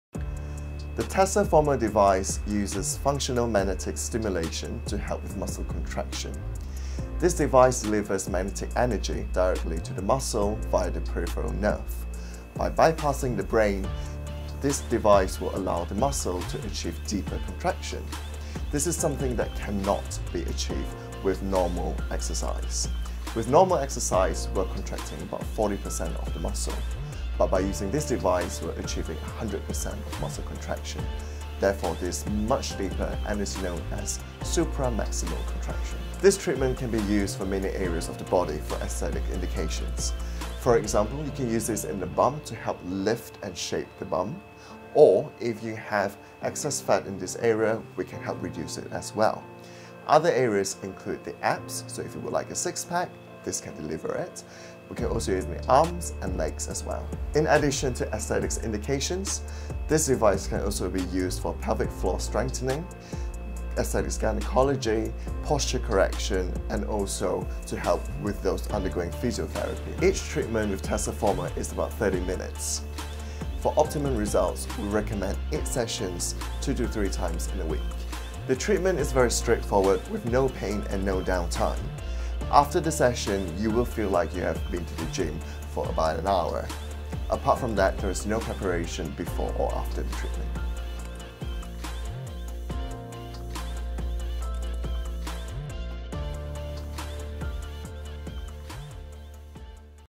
TeslaFormer | Dr Vincent Wong | What is TeslaFormer? | Build Muscle & Burn Fat
TeslaFormer uses FMS(Functional Magnetic Stimulation) technology which delivers magnetic energy (3 Tesla) to the peripheral nerve of the muscle in order for it to contract the muscles deeply, known as supramaximal contractions. The contractions you achieve with TeslaFormer is impossible to be achieved with a regular exercise. When you are exercising a muscle, the body will only allow you to contract that muscle to %40 of its maximum potential.

TeslaFormer bypasses the brain and contracts the muscle directly with the Functional Magnetic Stimulation technology. TeslaFormer, like an exercise, tears the muscle tissue. The body’s reaction to the damage is to repair. While the body repairs the muscle it also builds it.

IT CONTRACTS MUSCLE BUT HOW DOES IT TARGET FAT?

Your muscles store a bit of energy for a short period of time. When the muscle is contracting, within a few minutes it exhausts the energy it needs so it looks for fuel somewhere else and starts to draw on fat in the targeted area.

This is why, whether you are targeting the abs, glutes, thighs, calves, triceps & biceps, you will get that reduction in fat while building muscle.

NON INVASIVE BUTT LIFT – NON SURGICAL BUTT LIFT

TeslaFormer reduces the bum as it targets fat to build muscle. It is also perfect for toning and shaping droopy or flat bums.

THE TREATMENT
TeslaFormer contracts the muscles 50.000 times in a 30-minute session. We recommend 8 sessions in a 3-week period. Sessions can be carried out every other day, 3 sessions a week or a minimum of 2 sessions a week is recommended. The treatment is painless and without any needles. During the treatment, you will not need to remove your clothes, as magnetic field penetrates through clothes.